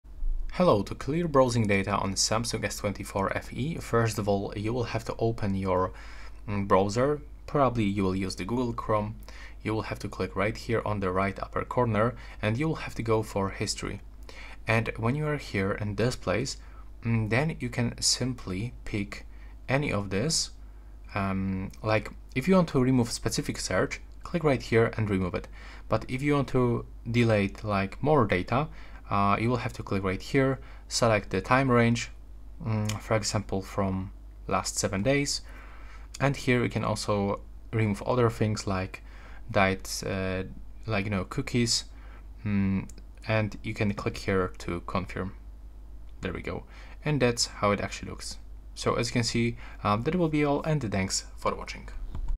Samsung S24 FE: How to Clear Browsing Data?
This video will show you how to clear browsing data on your Samsung Galaxy S24 FE. I’ll guide you through the steps to remove your history, cookies, and cache, helping to free up space and enhance your browsing experience on your Samsung S24 FE. Whether you're looking to maintain privacy or improve performance, this tutorial will help you easily clear browsing data on your Galaxy S24 FE.